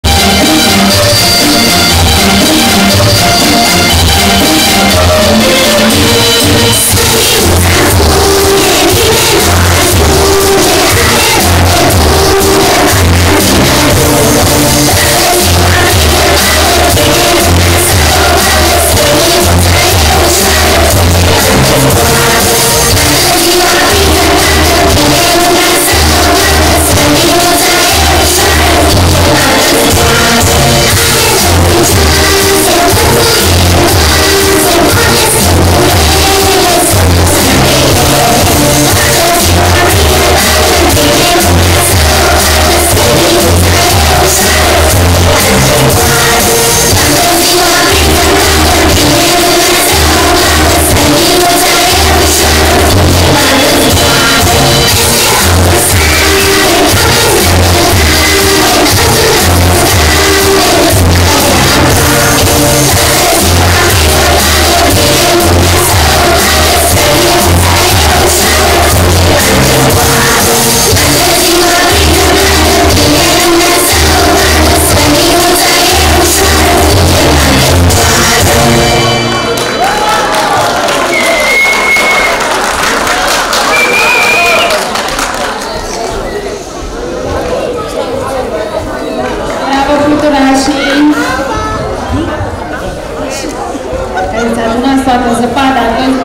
POVESTE DE CRACIUN 2019
Eveniment organizat de grupul de dans BOOM BOOM PANTELIMON.
Locatie: CASA DECULTURA A STUDENTILOR BUCURESTI
Data: 20 decembrie 2019